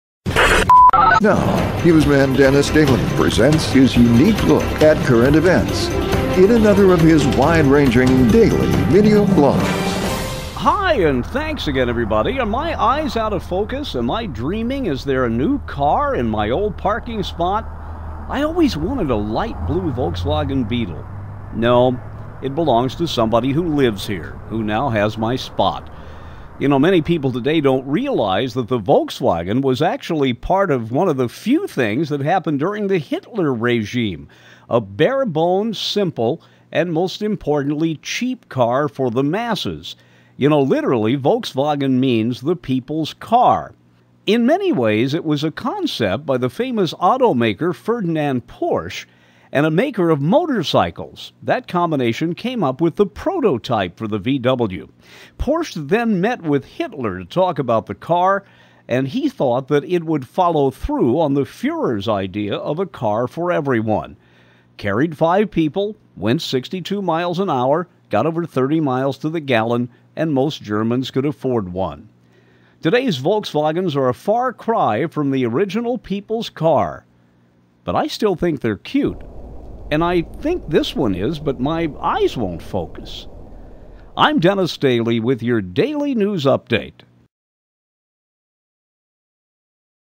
DAILY NEWS UPDATE 03-04-13 with Dennis Daily
IT WINS THE AWARD FOR BARE-BONES ... is the topic, as longtime national reporter Dennis Daily expresses his opinions on the passing scene.